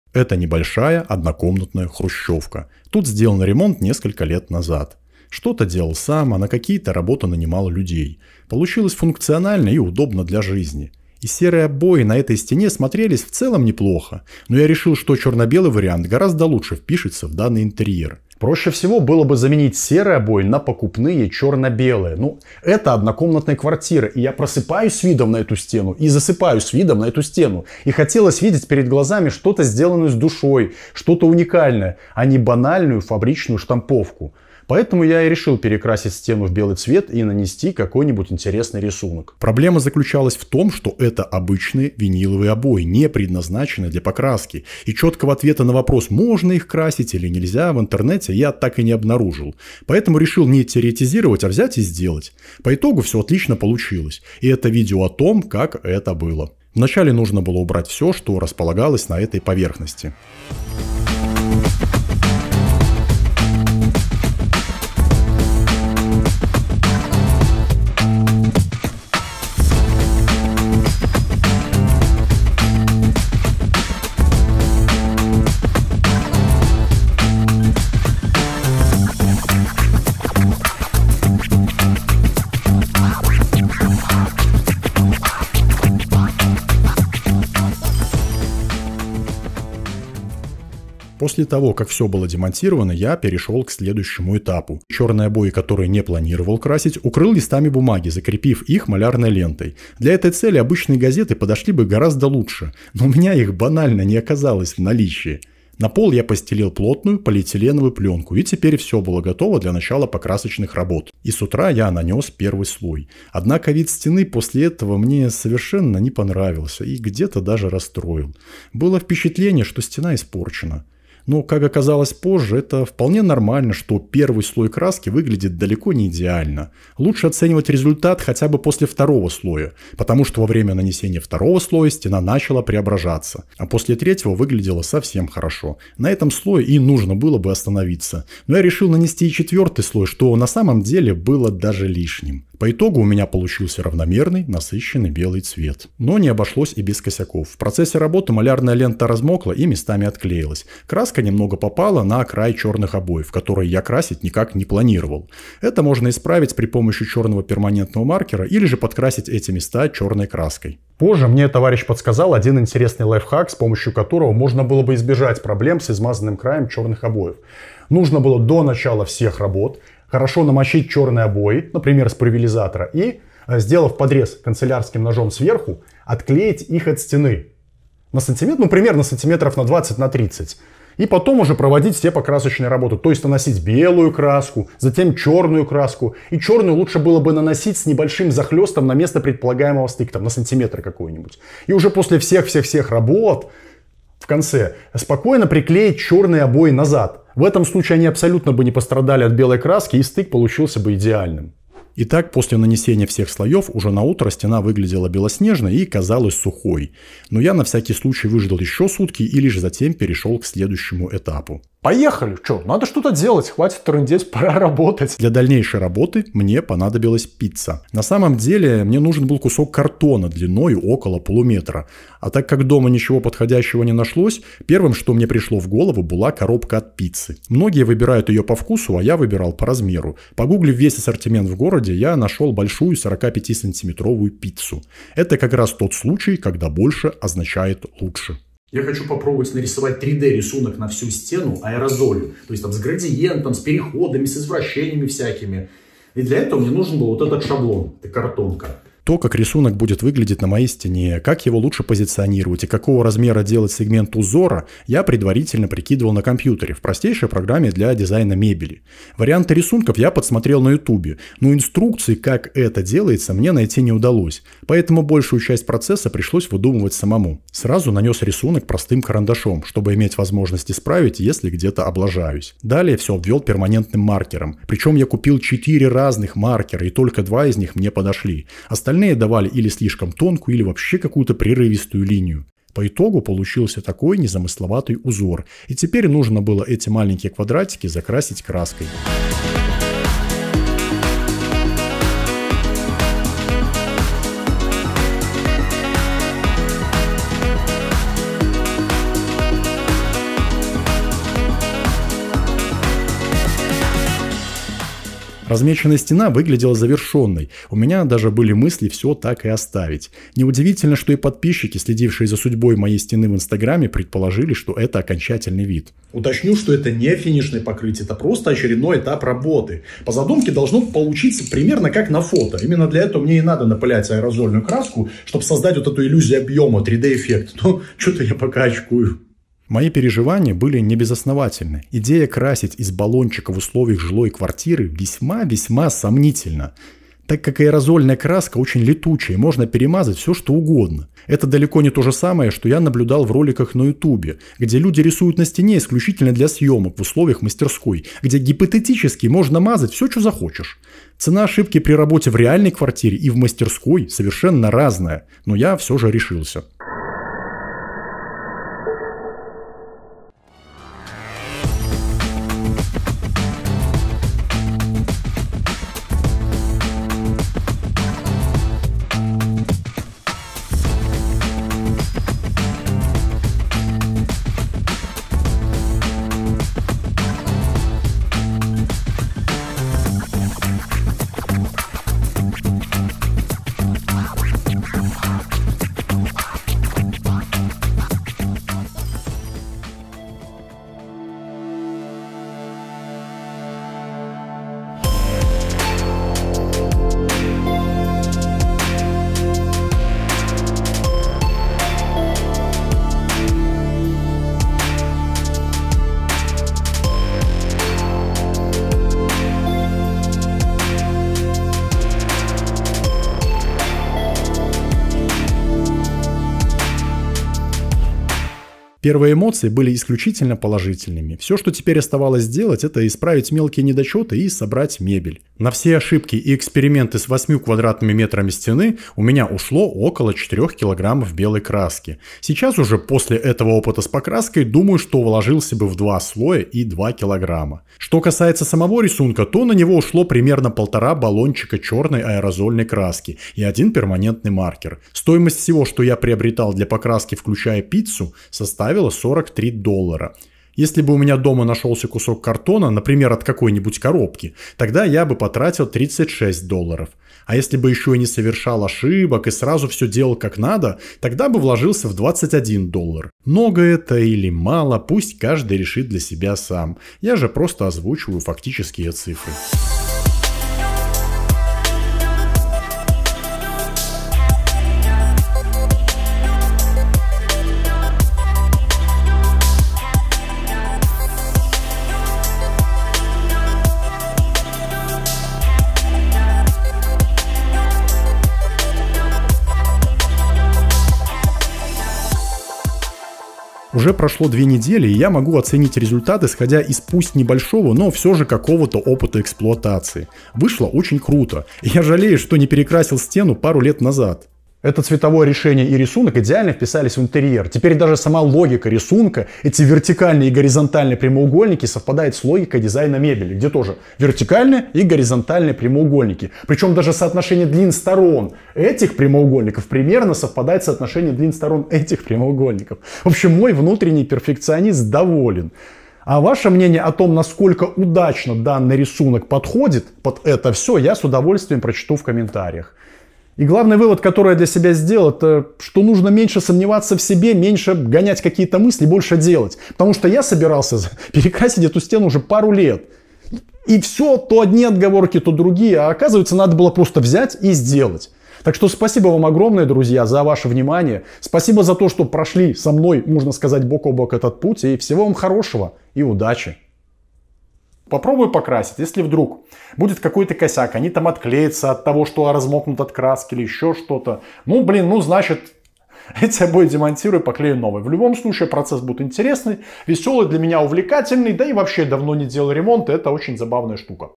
3D декор стен своими руками. Покрасить обои не под покраску.
Это небольшая однокомнатная хрущевка (28кв.м) в которой я живу последние несколько лет после того как тут был сделан ремонт. Что-то делал сам, а на какие-то работы нанимал людей. Получилось функционально и удобно для жизни.

Я сам рисовал дизайн-проект и в целом все получилось примерно как и планировал.
Уточню, что я не строитель и не дизайнер и делать что-либо своими руками просто мое хобби.

Изначально на стене была вставка из серых обоев. До того, как появилась мебель они смотрелись вполне неплохо. Но как только мебель стала на свои места стало понятно, что существует и более оптимальный вариант. Мысли заменить обои меня посещали несколько лет, но только теперь дошли до этого руки.

Проблема заключалась в том, что это обычные обои не предназначенные для покраски. Правильным было бы заменить их на обои под покраску и дальнейшие манипуляции проводить уже с ними. Но я решил попробовать покрасить сразу же эти обои избежав этим целого этапа работ с переклейкой обоев.

Знакомые строители утверждали, что так делать нельзя, что обои обязательно вздуются пузырями. А краска обязательно облезет и осыпется. Но какой-либо четкой аргументации, почему это должно произойти я так и не услышал.
Поэтому  решил не гадать, а взять и сделать.

И все хорошо получилось. Внешне мне теперь стена нравится гораздо больше чем раньше. И пока краска хорошо держится и не подает признаков, что ей что-то не нравится.

Постарался весь процесс показать максимально интересно и очень надеюсь, что видео вам понравится. Если у вас возникнут какие-либо вопросы буду рад ответить.